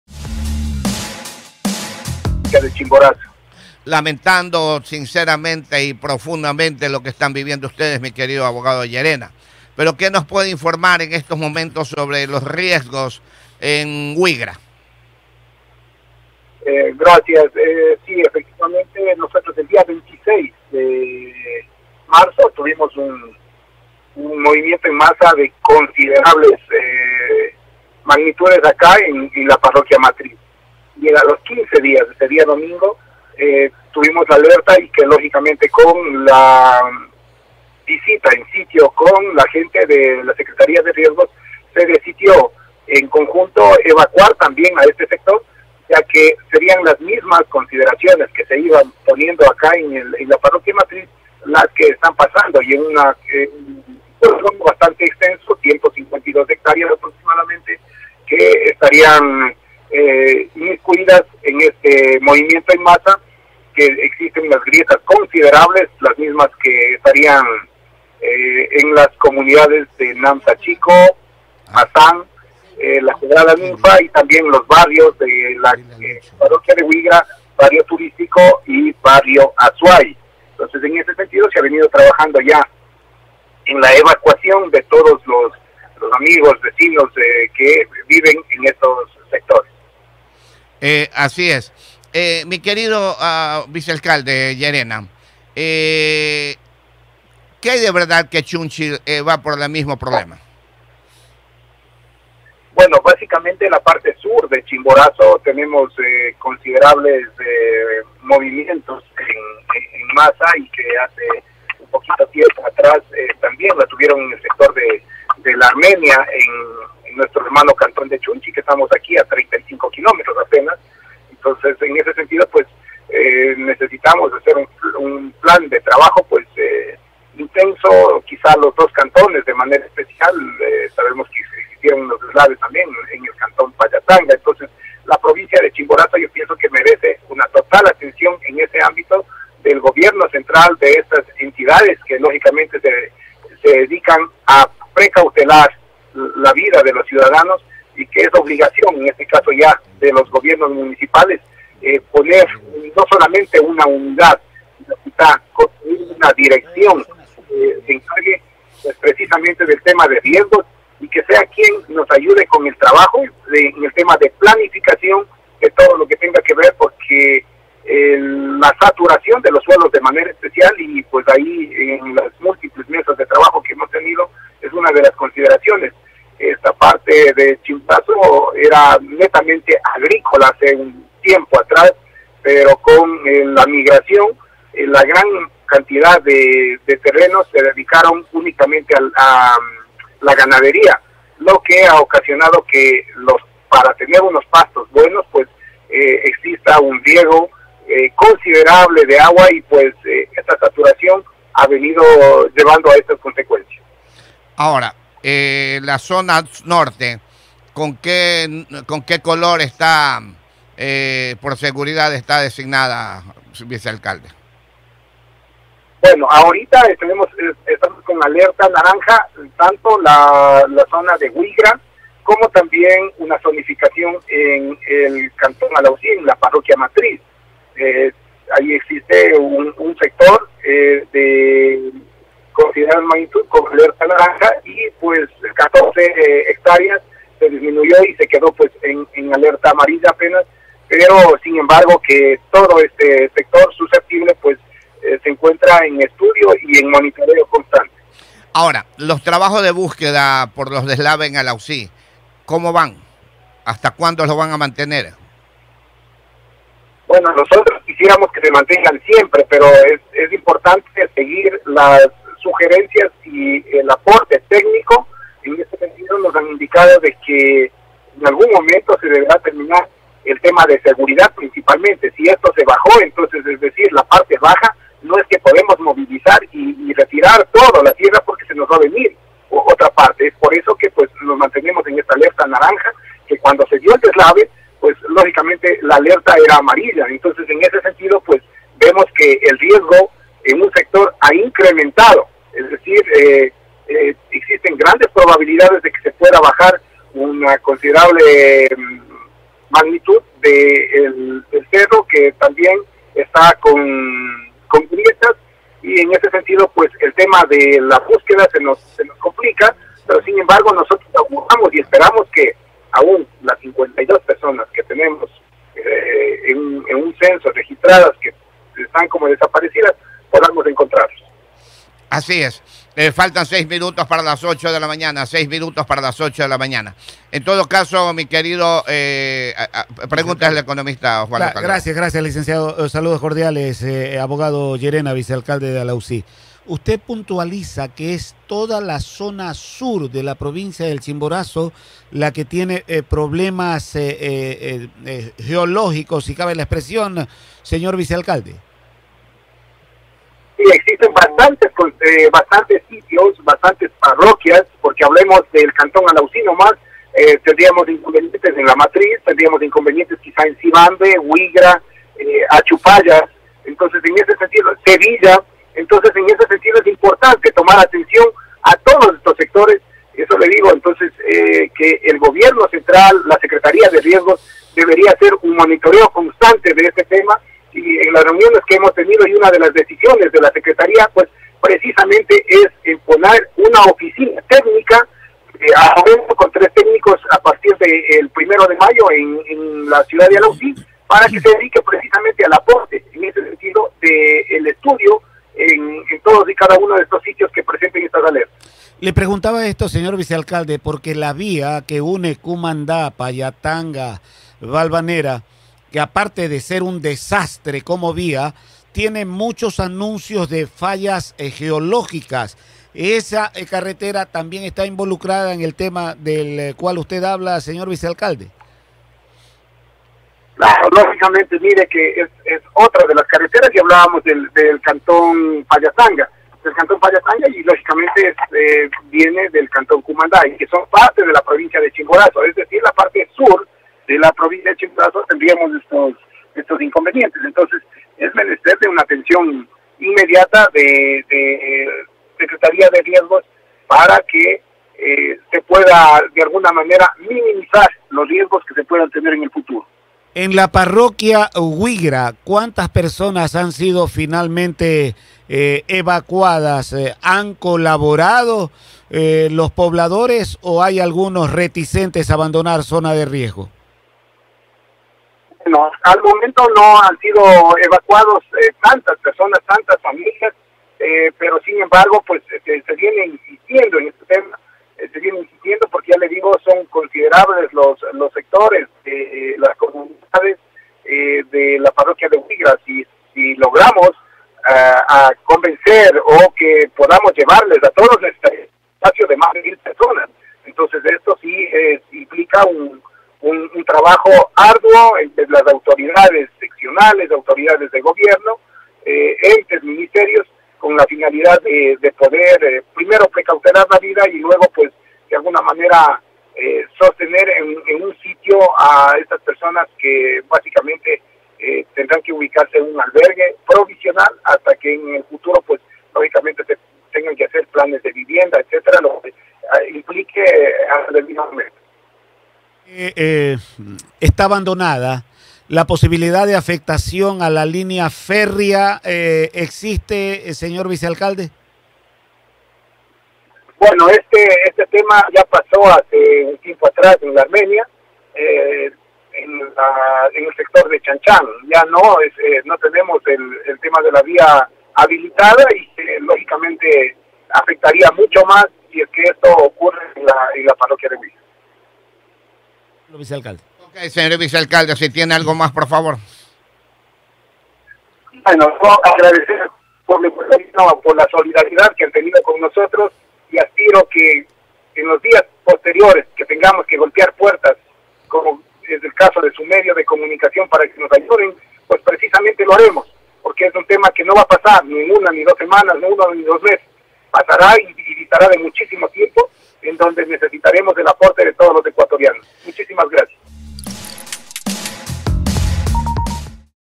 ABG. EDUARDO LLERENA VICEALCALDE DE ALAUSÍ
TEMA: * RIESGO EN LA PARROQUIA HUIGRA

* EVACUACIÓN DE POBLACIÓN

* TRABAJOS DE BUSQUEDA EN DESLAVE DE ALAUSÍ